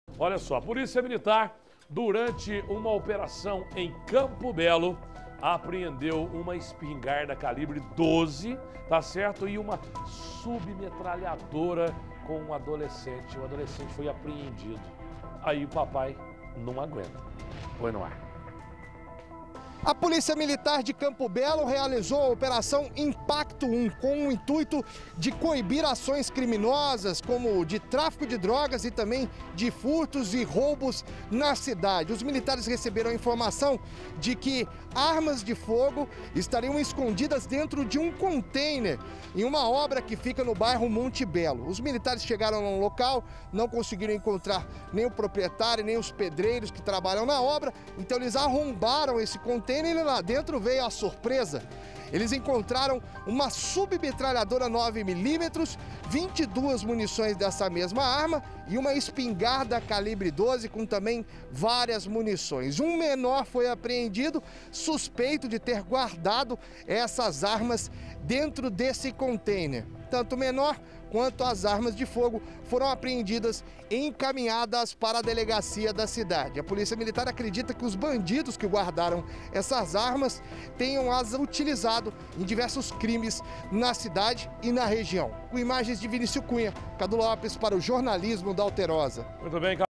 Armas apreendidas em Campo Belo
ARMAS APREENDIDAS EM CAMPO BELO | Armas de fogo foram apreendidas dentro de um container.